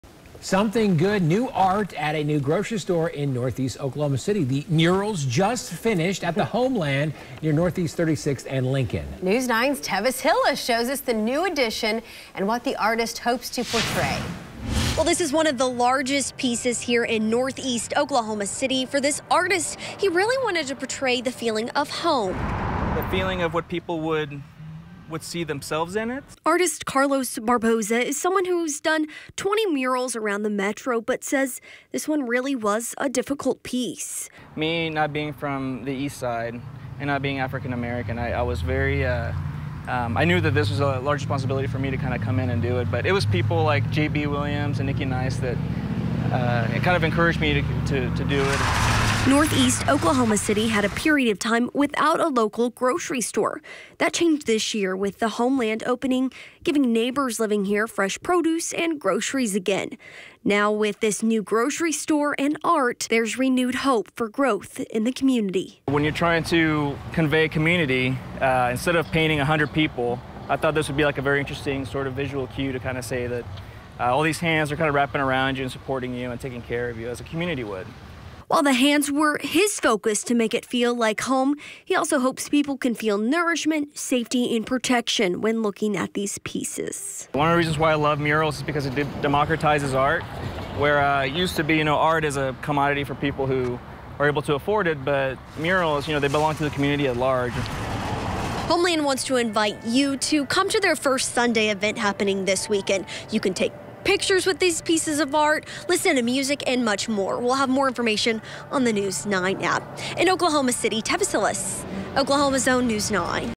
Artist Puts Finishing Touches To New Murals At Homeland In NE OKC
At Homeland on Northeast 36th and Lincoln, one of the largest art projects is nearing completion. The artist Carlos Barboza wanted to portray these three panels as “home.”